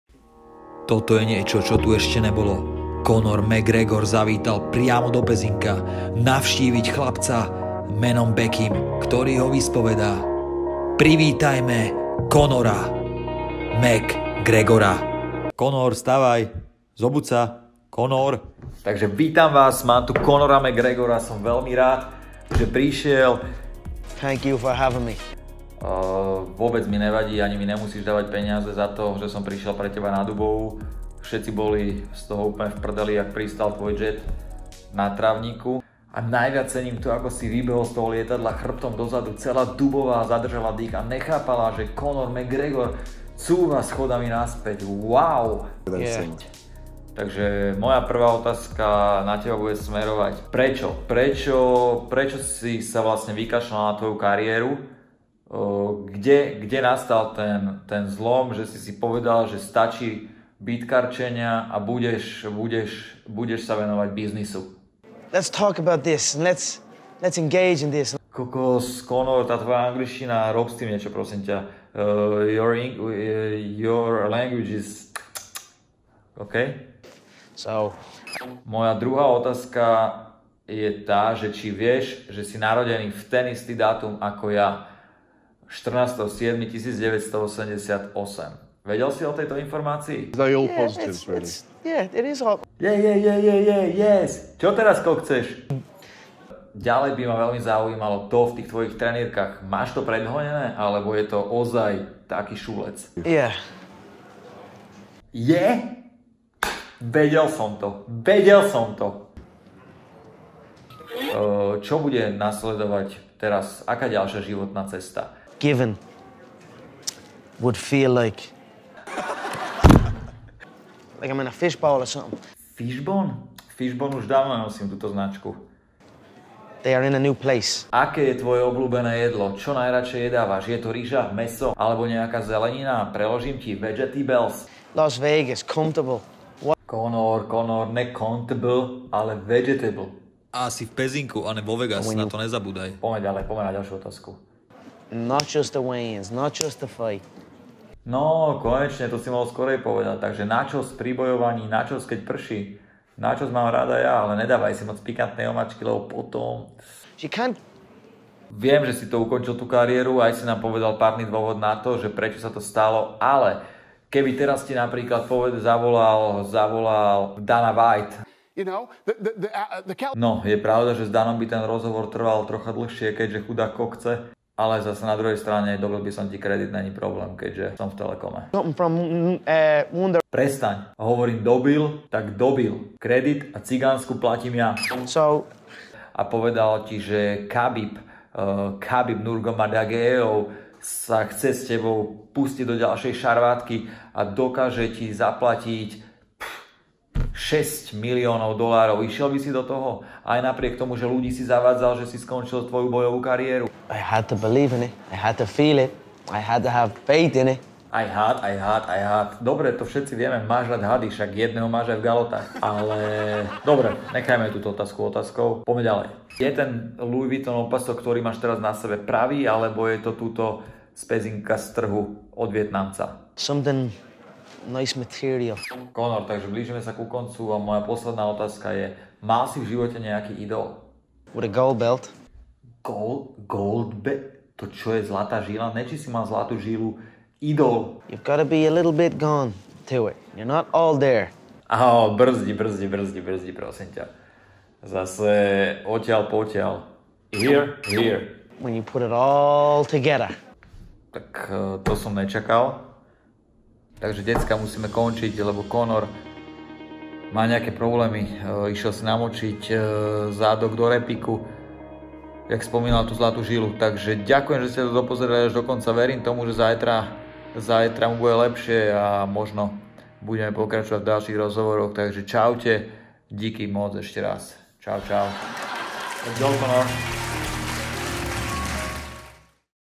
PREČO CONOR MCGREGOR UKONČIL KARIÉRU? (vyspovedal som ho)
Dosť bolo rečí!! Tu je pravý dôvod PREČO Conor McGregor skončil s EMEMEJ.